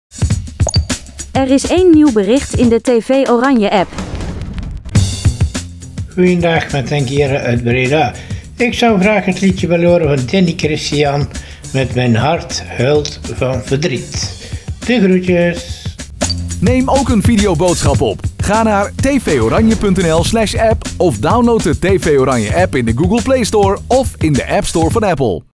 TV Oranje app videoboodschap - Henk Heeren
Via de TV Oranje app stuurde Henk Heeren een videoboodschap in. Wil je ook een videoboodschap inspreken? Download dan nu de TV Oranje app op je telefoon of tablet